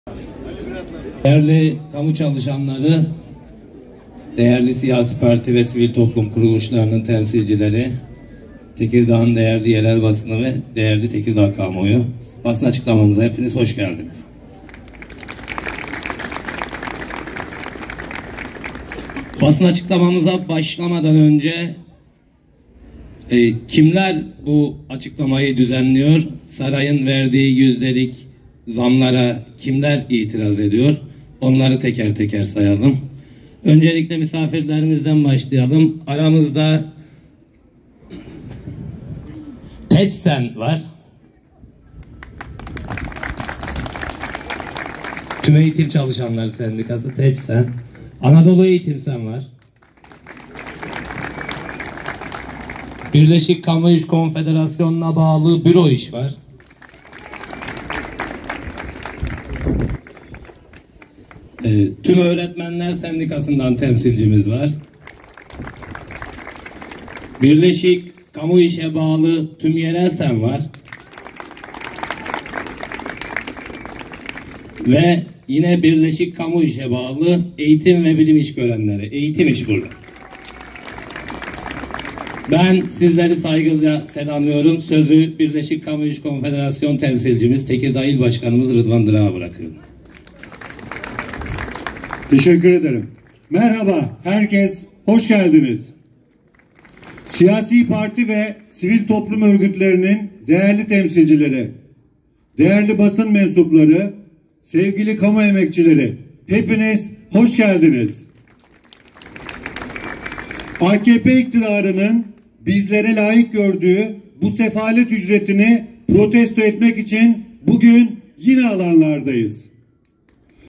KAMU EMEKÇİLERİ İNSANCA YAŞAM İÇİN BİR GÜNLÜK İŞ BIRAKTI
Birleşik Kamu-İş Konfederasyonu Tekirdağ İl Başkanlığı, insanca yaşabilecekleri ücret için bir günlük iş bırakma eylemi gerçekleştirdi. Memur ve emekli maaşlarına verilen yüzde 30 zamma tepki olarak yapılan iş bırakma eyleminde, kamu emekçilerinin ve tüm emeklilerin maaşlarına en az asgari ücret artış oranı kadar zam yapılması talep edildi.